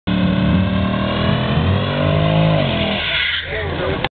Charger SRT-8 Warmup Burnout
Charger SRT-8 (6.1L) doing a warmup burnout at Rockingham Dragstrip on 3/13/2011 for Phantasm Street Wars 1